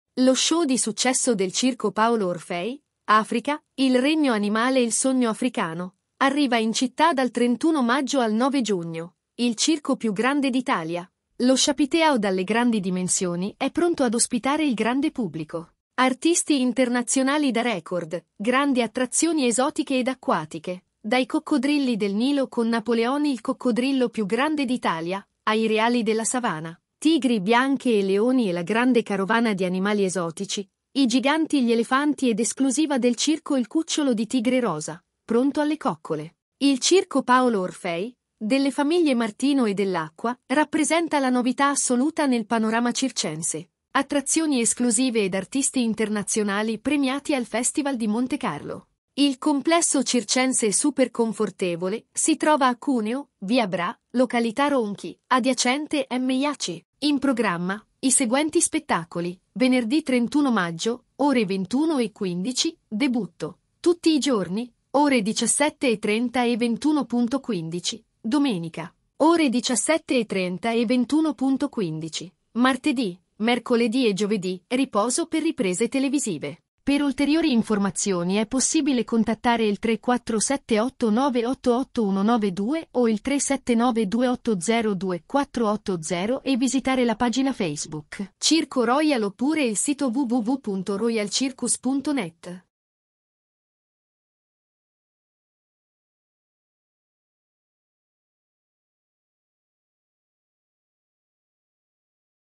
Cuneo: lo show di successo del Circo Paolo Orfei, “Africa, Il Regno Animale”
Il canale ufficiale degli appassionati del circo